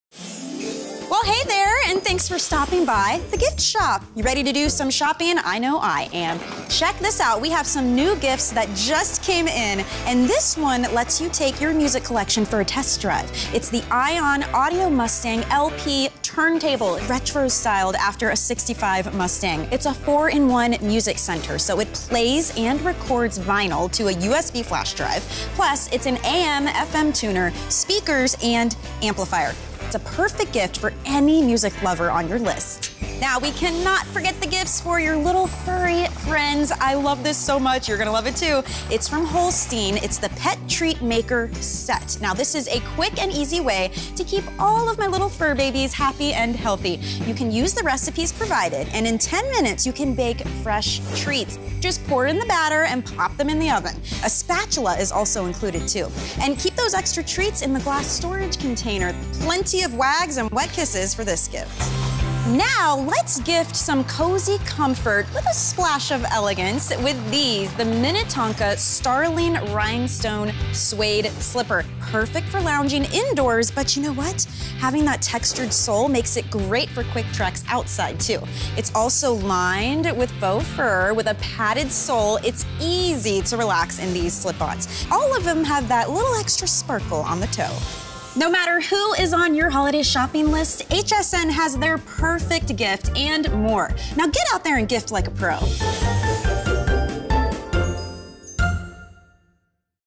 Gifts for him, her and even pets
Michelle Yarn, HSNs Gift Guru, shows you perfect gifts for every person...and furry friend...on your list This week, see gifts from Minnetonka, Holstein and Ion Audio. Shop more great gifts in the HSN Gift Store https